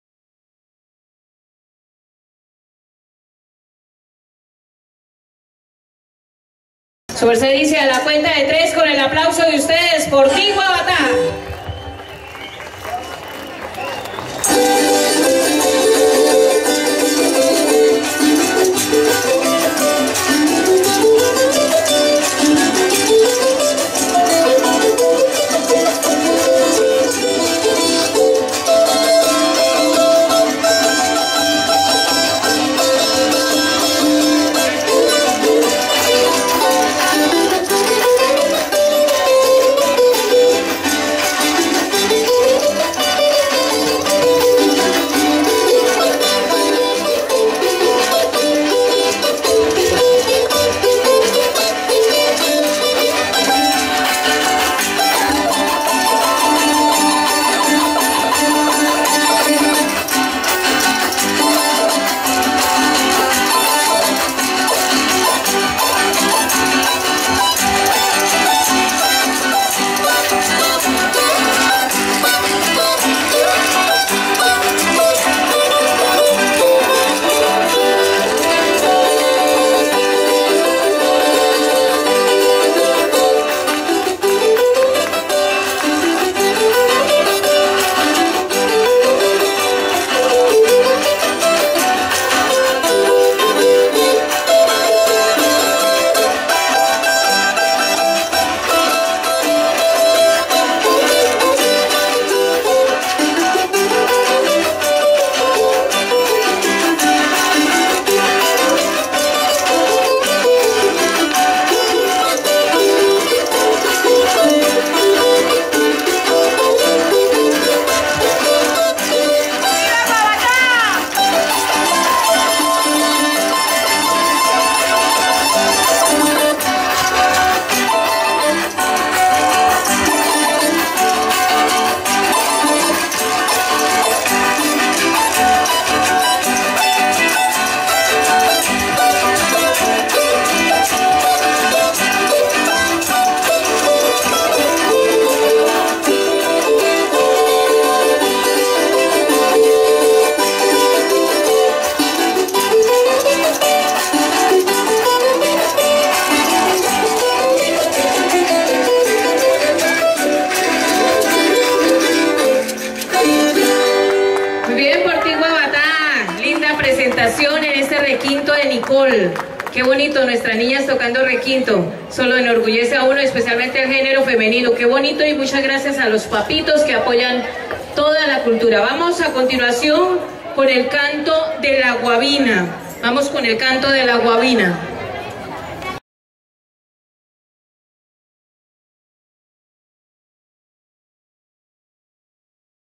TORBELLINO, FERIAS PUENTE NACIONAL
Con orgullo y alegría compartimos con ustedes algo de lo que se vivió en las ferias de Puente nacional, Santander donde Guavatá hizo presencia enalteciendo nuestra cultura y folclore